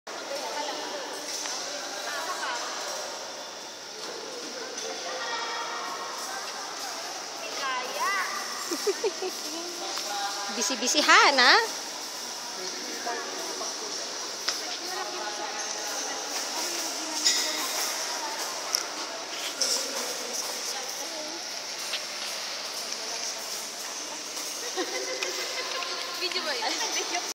Ocean Park (DadFred's Birthday)
Date Taken: January 04, 2014
Celebration held at H2O Bar then followed by Harbour Square Starcks

...Video taken after dinner celebration located at the main lobby of Ocean Park. ^_^